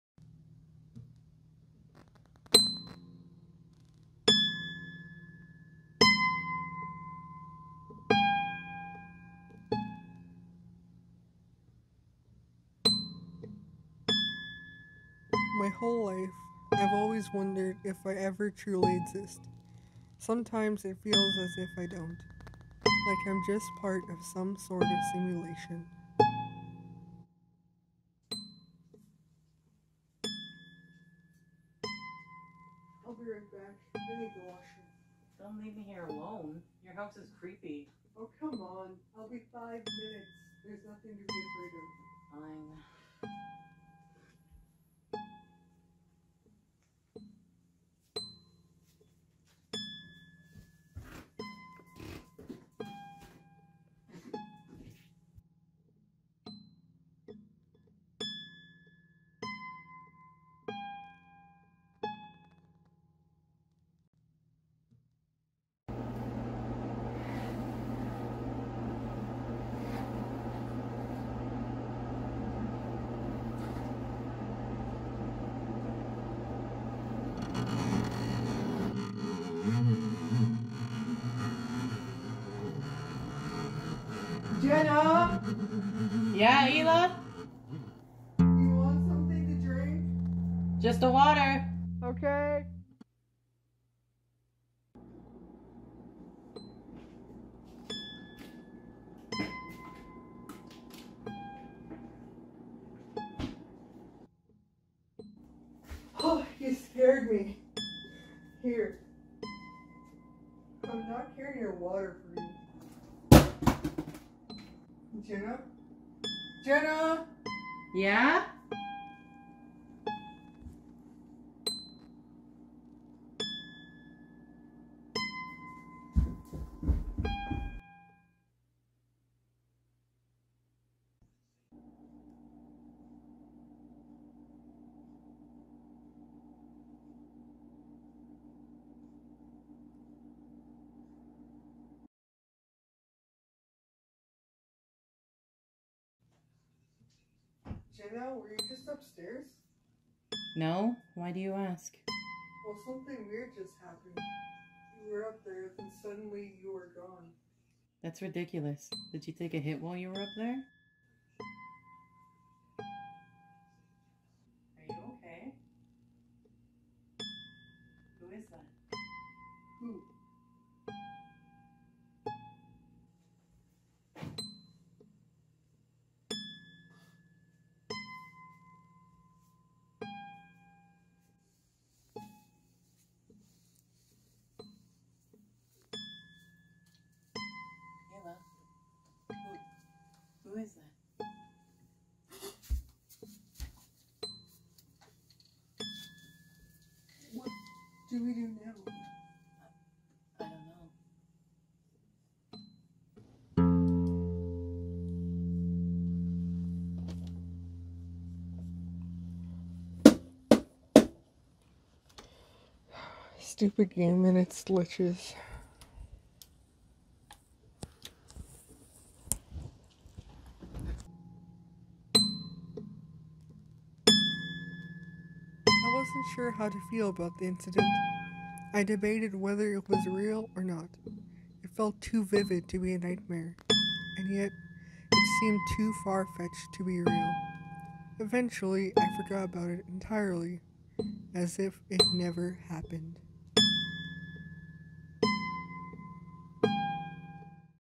Glitch - Short Horror Film
Original upload date: October 31st 2021

Original description: A Horror/Comedy short film by me and my friends. It's a project we've been working on for awhile and I'm happy to say we've finally finished it.